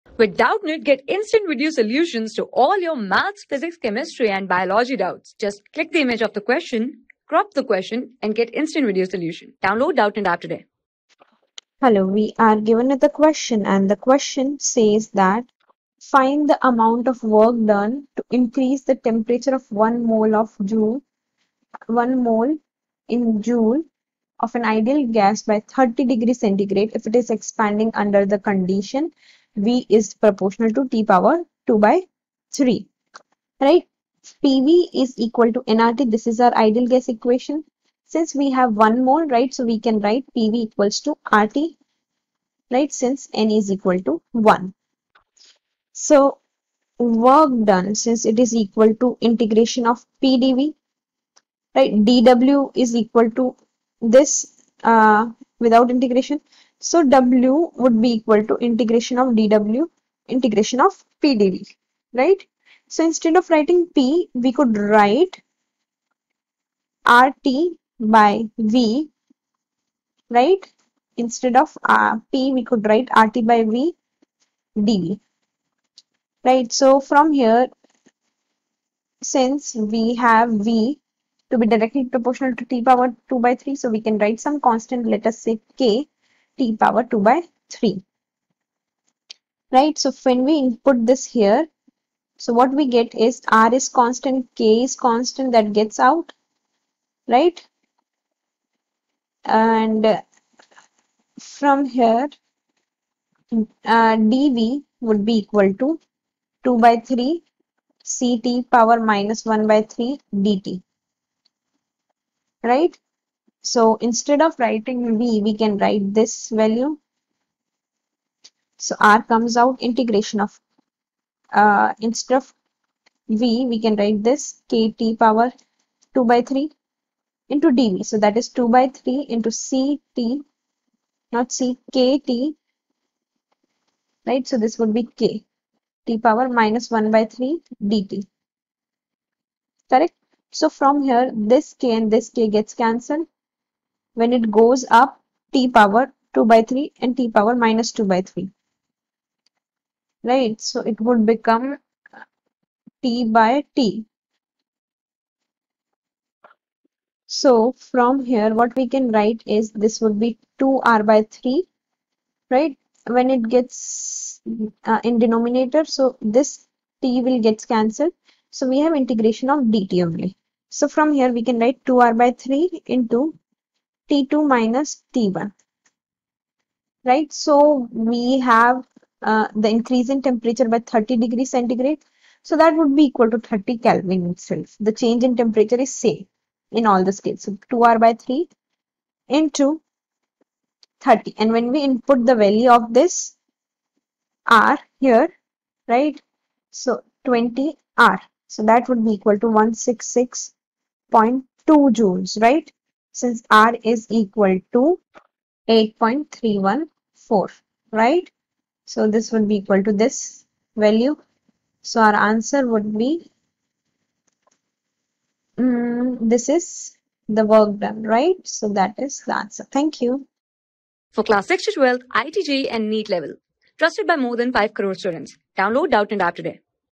Find the amount of work done to increase the temperature of one mole jof an ideal gas by 30^(@)C...
Find the amount of work done to increase the temperature of one mole jof an ideal gas by 30^(@)C, if it is expanding under condition VooT^(2//3).
Class: 12
Subject: PHYSICS
Chapter: GASEOUS STATE & THERMODYNAMICS
Board:IIT JEE

👉 Have Any Query? Ask Us.